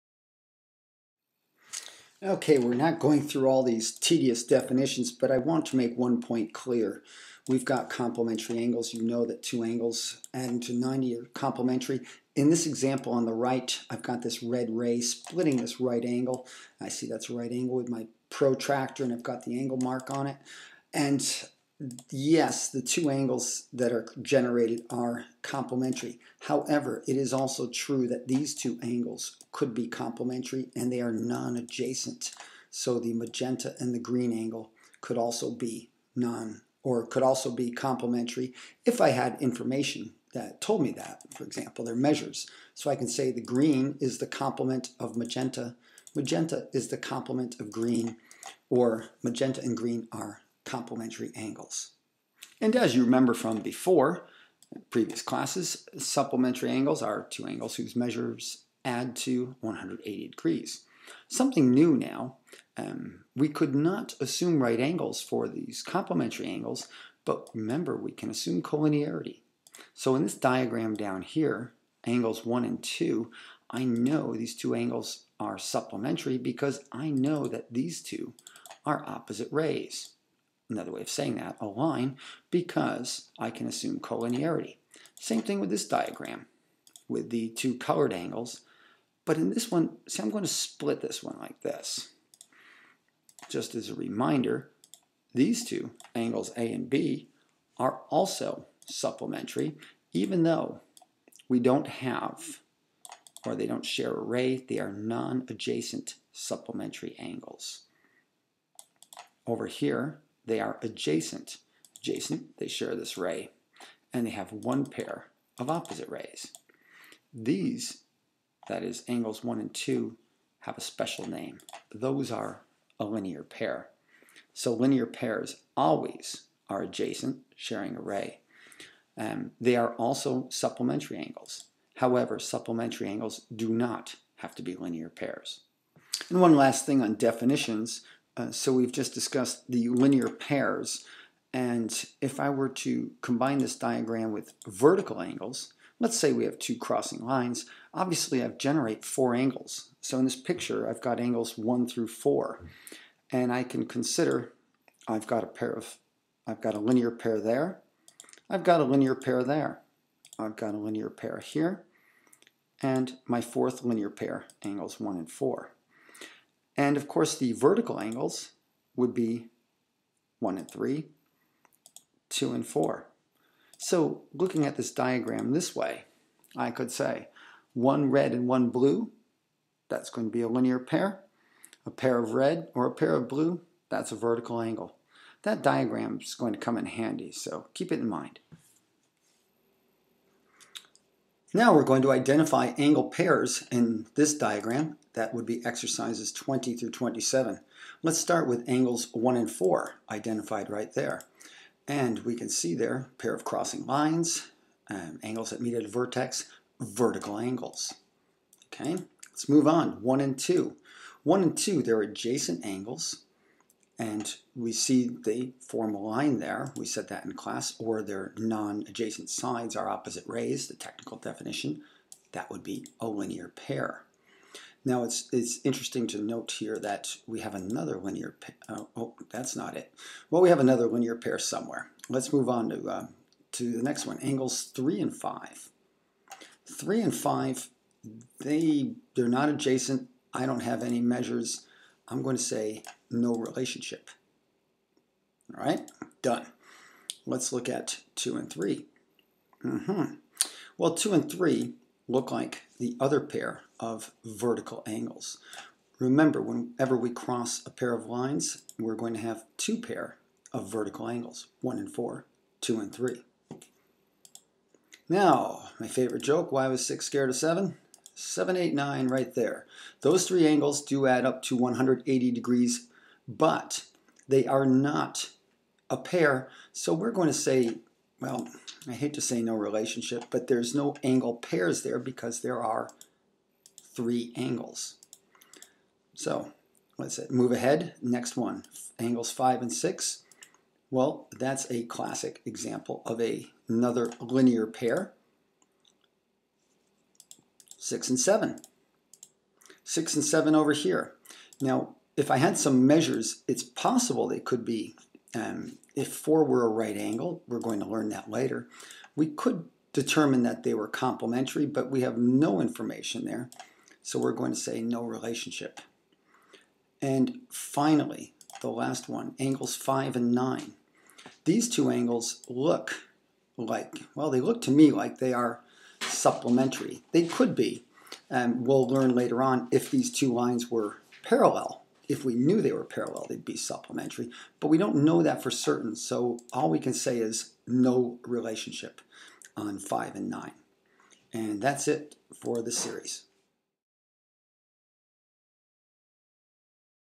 1.5 Define & recognize angle pairs
Here we define complementary angles, supplementary angles, linear pairs, and vertical angles.  Exercises 20-27 give us practice in recognizing these special angle pairs in a more complex drawing.